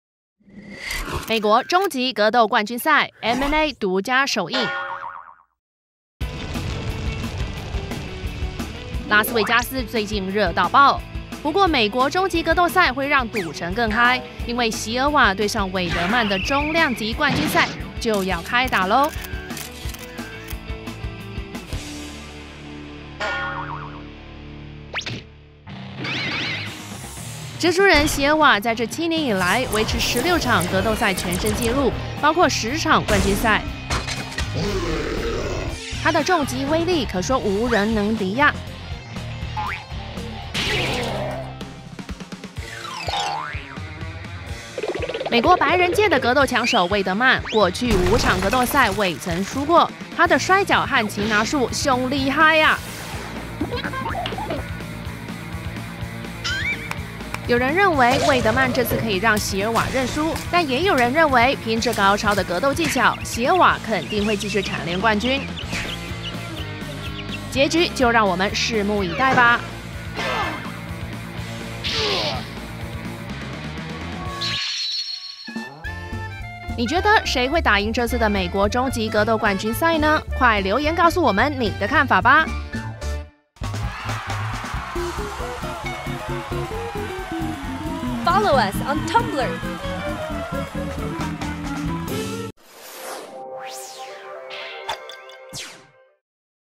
UFC 162 Silva vs Weidman: MMA gets all NMA
UFC 162 is almost upon us and everyone has their eyes on the middleweight title fight between champion Anderson Silva and top contender Chris Weidman.